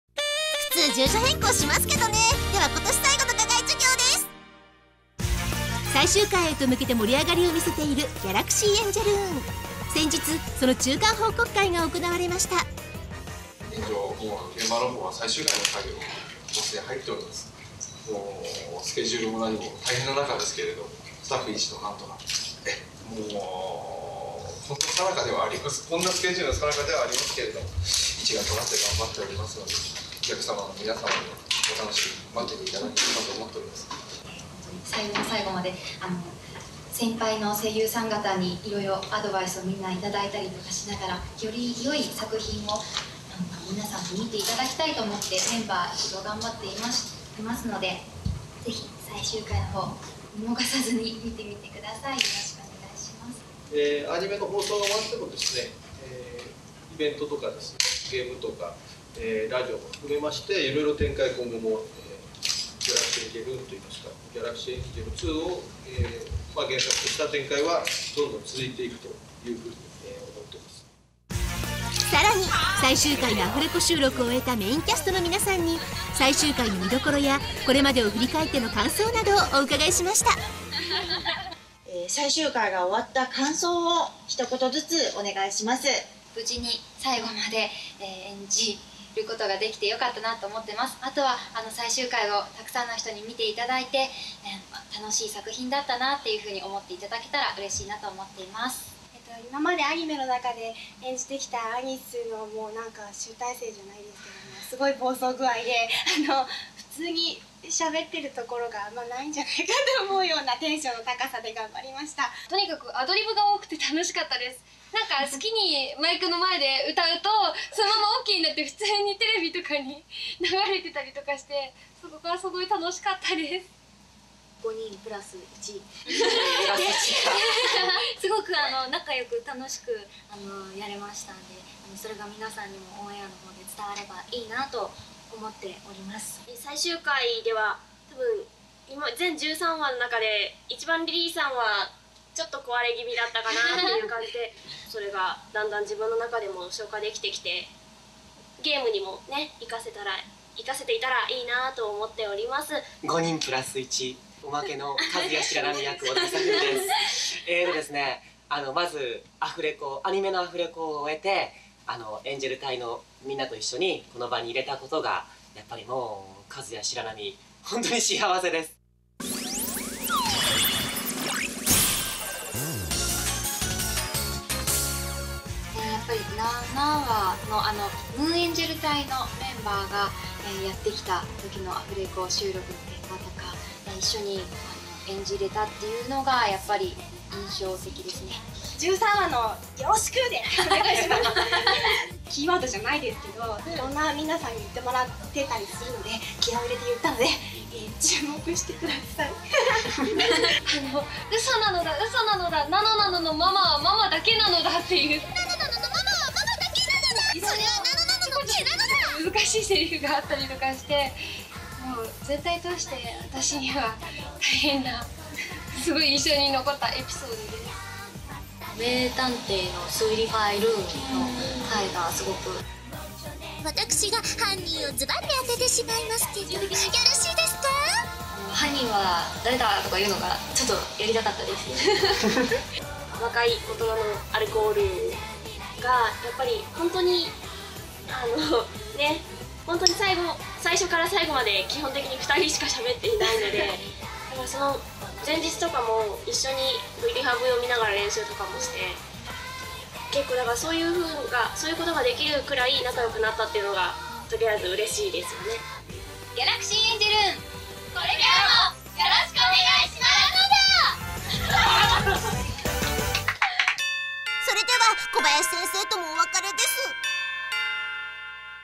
Galaxy Angel Rune: Mid-Season Report & Finale Post-Recording
Scenes from the presentation/press conference held at the midway point of the season and from the party celebrating the completion of the final episode.  Features the seiyuu Inamura Yuuna (Apricot), Hanamura Satomi (Anise), Akesaka Satomi (Nano-Nano), Hirano Aya (Kahlua/Tequila), Nakayama Erina (Lily), and Oda Hisafumi (Kazuya Shiranami), who totally looks like Suzumura Kenichi...